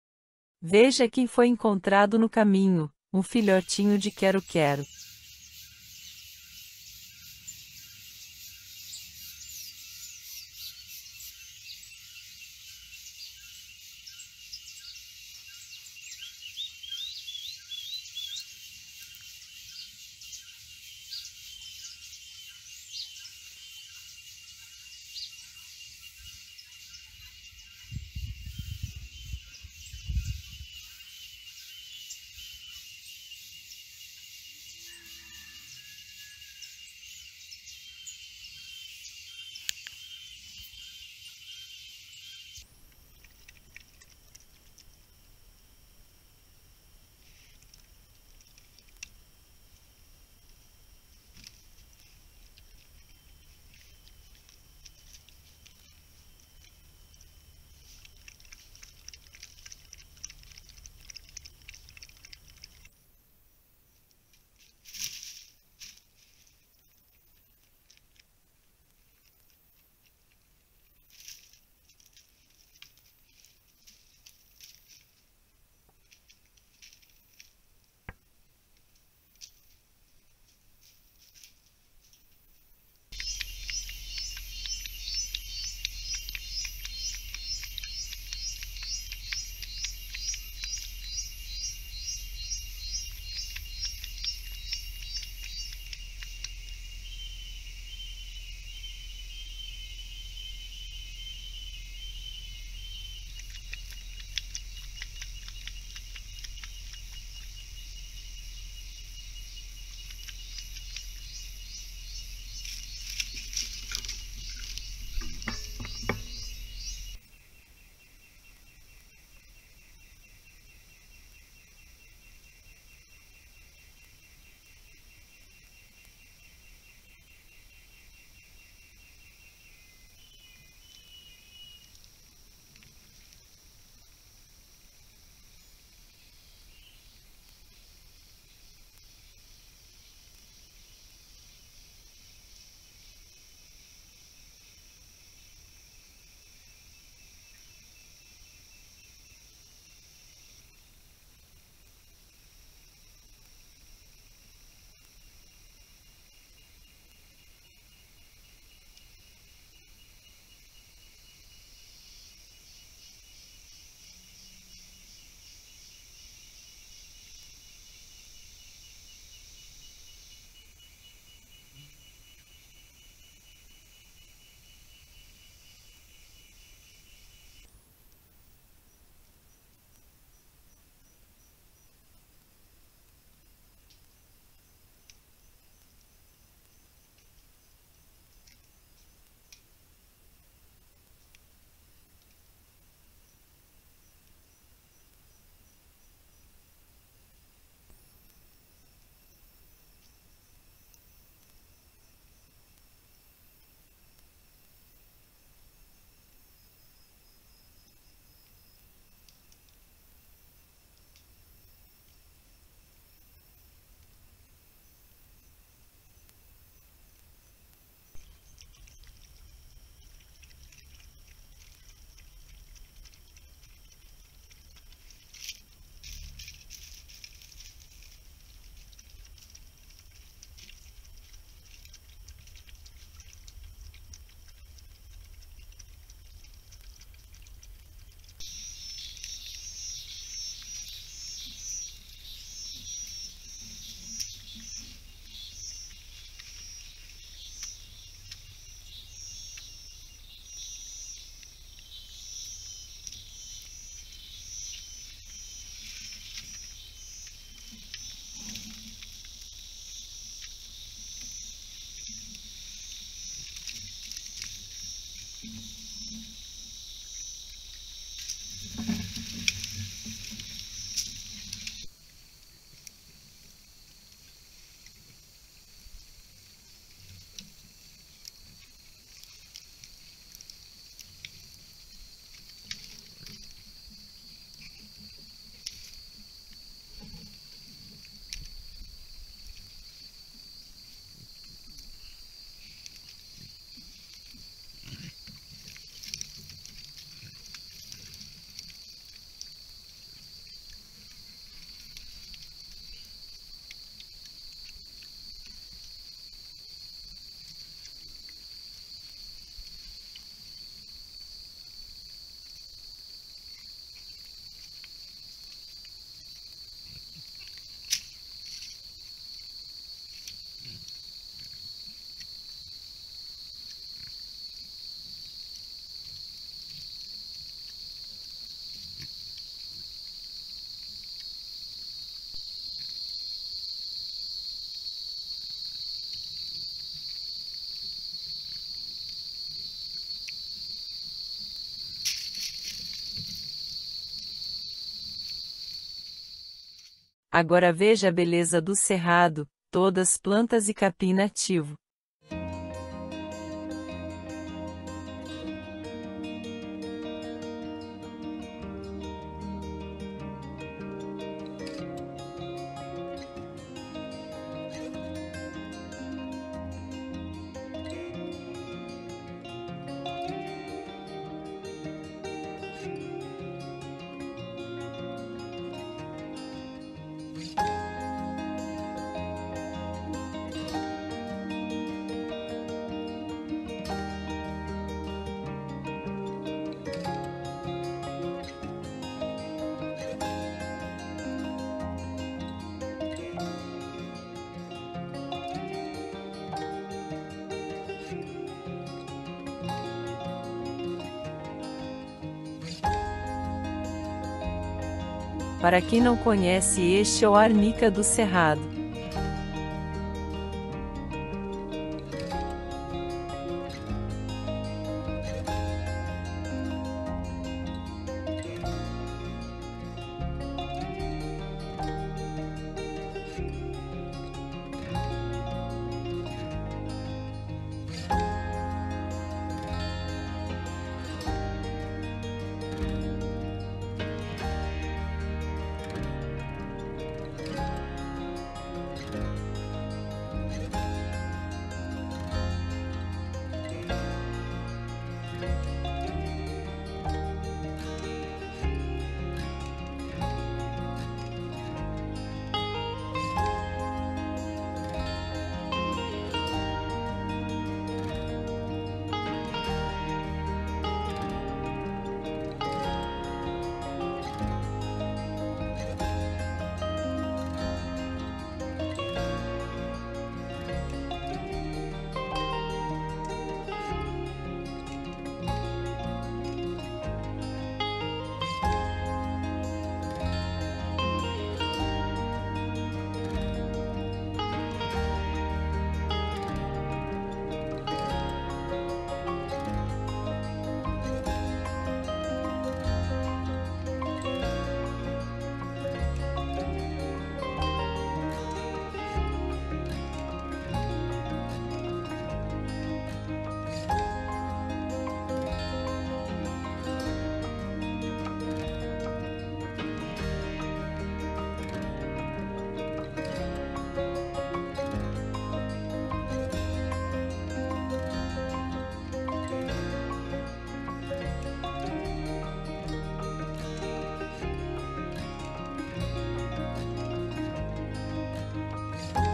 muitos animais tomando água
muitos animais tomando água no bebedouro